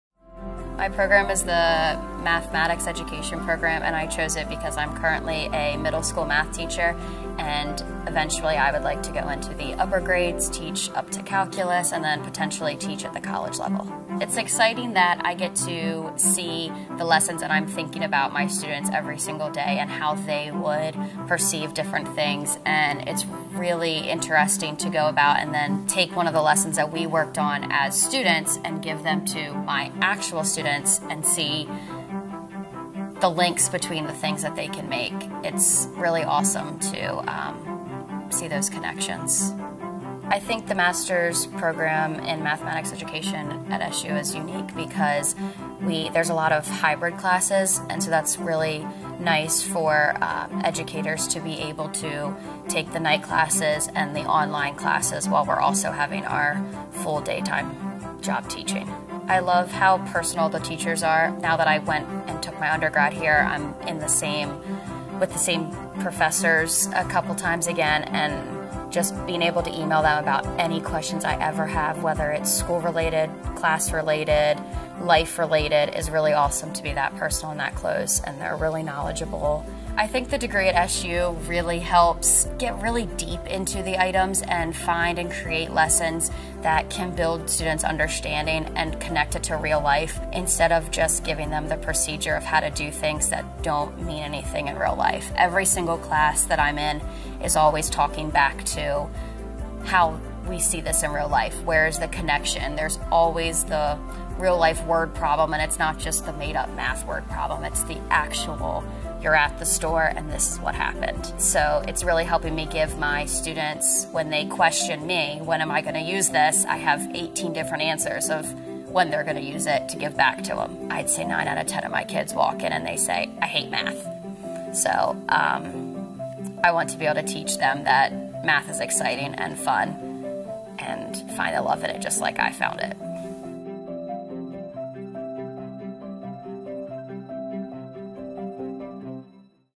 Meet Karli – M.S. in Mathematics Education Student at Salisbury University
Hear how Karli’s classes help her teach her students real-world applications of math.